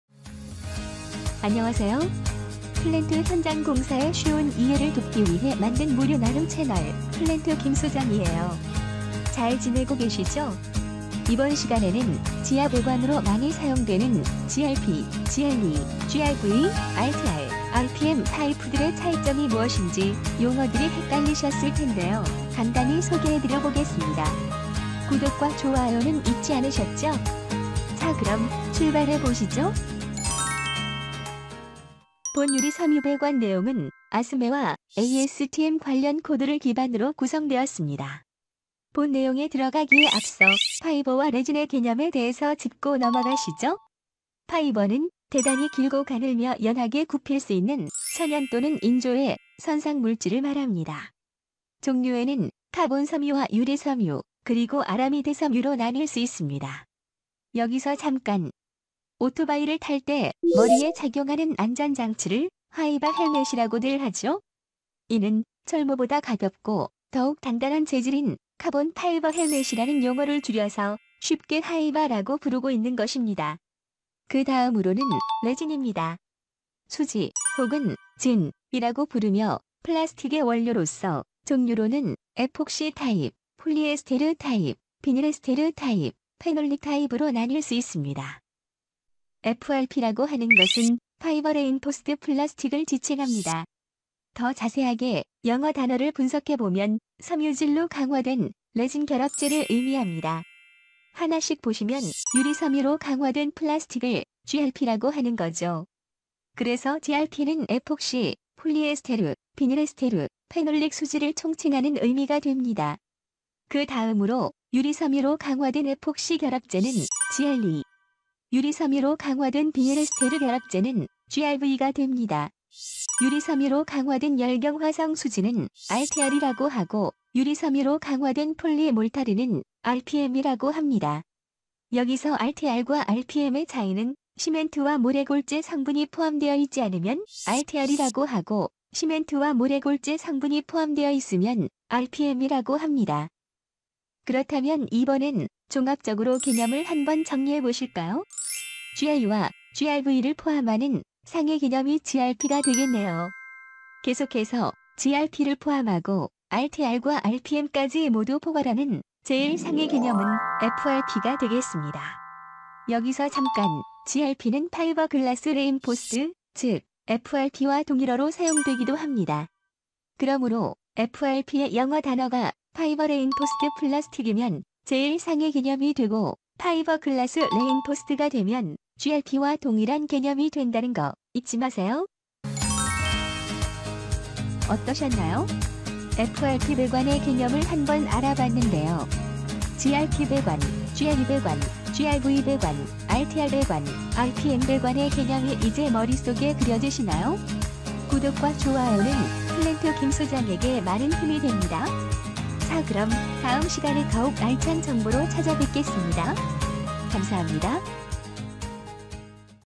🧠 GRP? GRE? RPM? 차이 모르고 시공하면 대형사고!💡 3분만에 정리되는 FRP 배관 계보도📘
📌 안녕하세요, 플랜트 김소장입니다 👷‍♂️
이번 영상에서는 플랜트 지하배관에서 자주 등장하는 FRP, GRP, GRE, GRV, RTR, RPM 등 복잡한 용어들을 아주 쉽게! 정리해드립니다. 도면, 발주, 시공, 검사까지 모두 연결되는 핵심개념, 3분 안에 뽀개드립니다.

📐 배관 재질 선정에 어려움을 겪는 초보 플랜트 시공 관리자
📋 도면이나 발주서에 FRP 관련 용어가 많아 헷갈리는 설계 엔지니어
🔧 현장에 처음 투입된 플랜트 배관 감리/검사자
🧑‍🏫 플랜트 전공 실무자 및 국가기술자격 준비생

🔍 FRP 배관 계보 한눈에 보기

FRP (Fiber Reinforced Plastic): 섬유 강화 플라스틱의 최상위 개념
GRP (Glassfiber Reinforced Plastic): 유리섬유 강화 플라스틱 = FRP와 동의어처럼 사용되기도 함
GRE (Glassfiber Reinforced Epoxy): 에폭시 수지 기반 GRP
GRV (Glassfiber Reinforced Vinylester): 비닐에스테르 수지 기반 GRP
RTR (Reinforced Thermosetting Resin): 열경화성 수지 기반 GRP 배관, 골재 없음
RPM (Reinforced Polymer Mortar): 골재(모래+시멘트)가 포함된 GRP 배관

🧪 핵심 용어 설명

Fiber: 구조강화용 섬유 (Glass, Carbon, Aramid)
Resin: 결합재 역할 수지 (Epoxy, Polyester, Vinylester 등)
Aggregate: 골재 (RPM에 포함됨, ASTM C33 기준)

📌 배관재 선택 실무 팁

GRE: 부식 환경, 해수라인, 케미컬 이송에 적합
GRV: 고온 고부식 환경에서 사용
RTR: 일반 UG(지하) 배관
RPM: 모래·시멘트 포함된 구조용 배관 (비용↓, 강도↑)
발주 사양서나 도면에서 용어가 혼용되므로, 반드시 정확히 파악 필요!